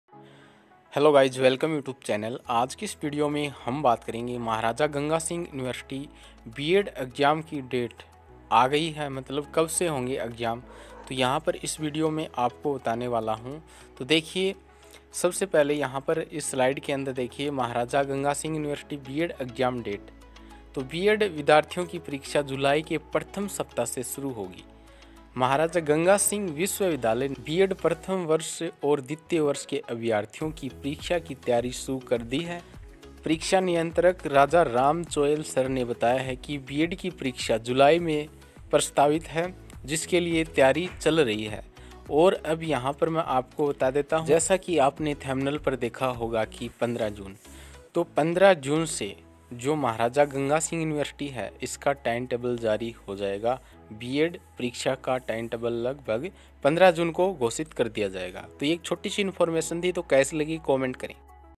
महाराजा गंगा सिंह यूनिवर्सिटी B.Ed Exam | MGSU B.Ed. Exam Date | बीएड परीक्षाएं कब होगी ?rjgkankit
महाराजा गंगा सिंह यूनिवर्सिटी | MGSU B.Ed. Exam Date | एमजीएसयू बीएड परीक्षाएं कब होगी ?mgsubikaner